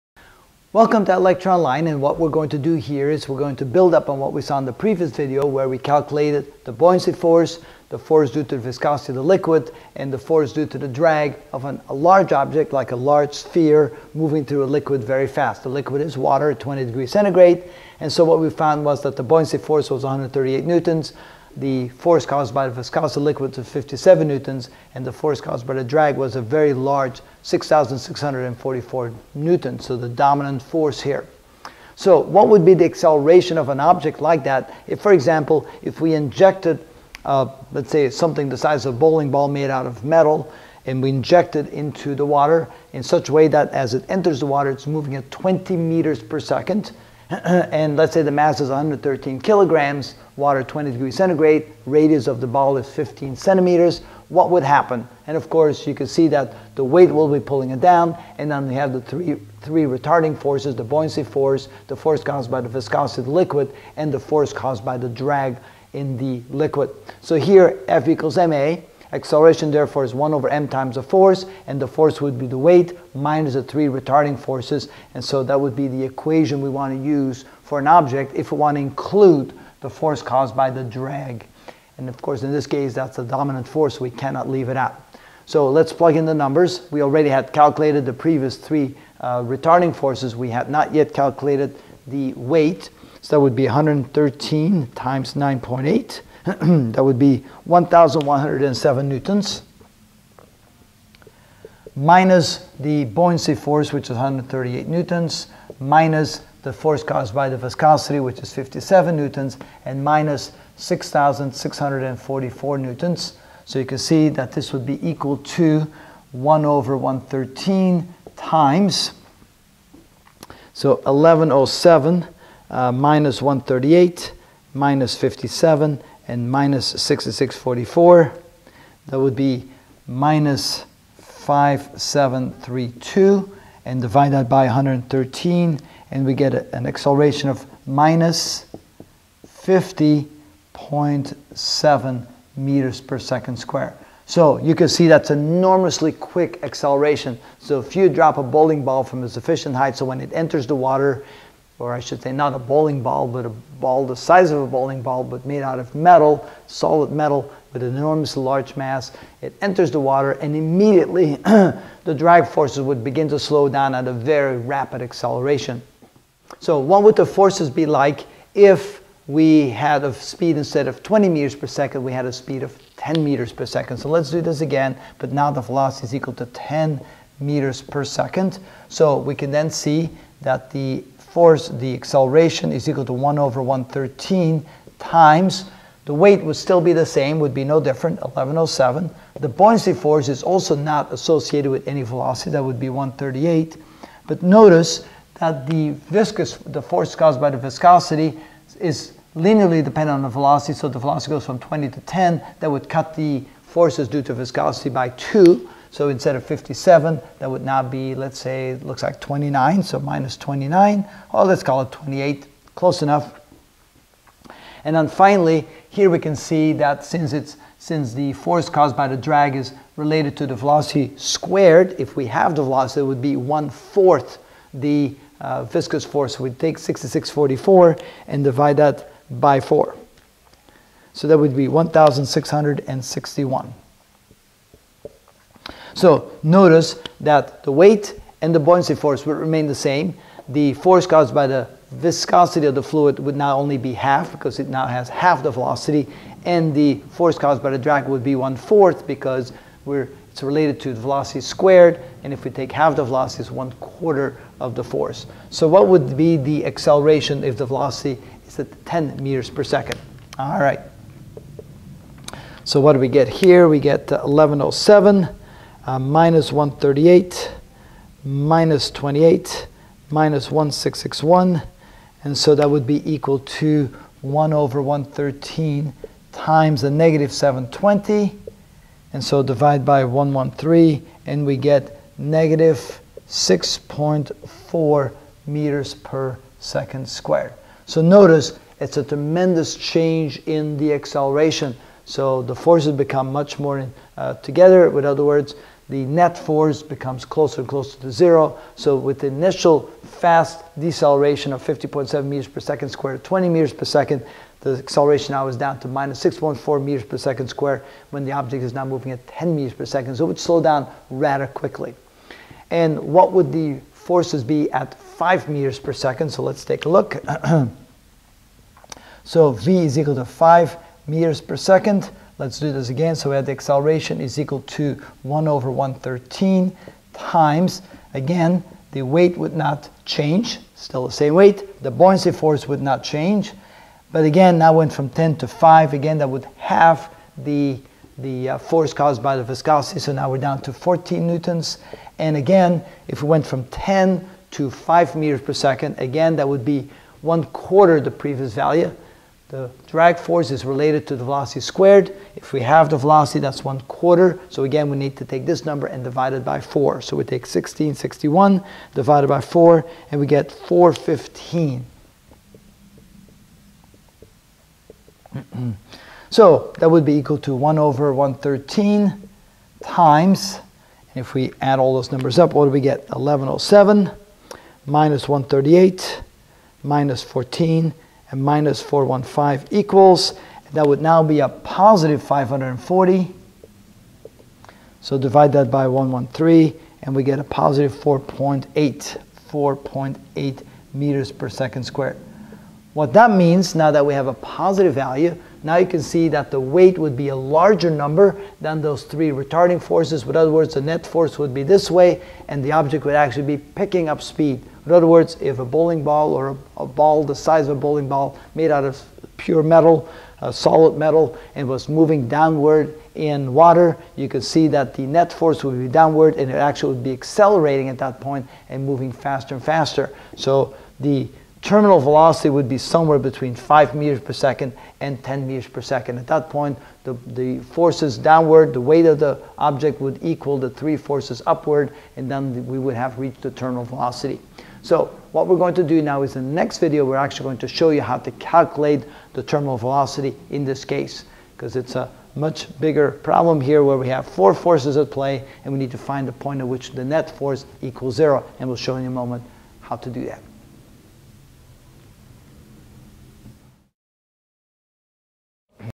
Physics 34 Fluid Dynamics (23 of 24) Buoyancy, Viscosity, and Drag Forces Compared: Trial 3
In this video I will find a=? of a metal ball of R=15cm, m=113kg sinking at v=20,10, and 5 m/s.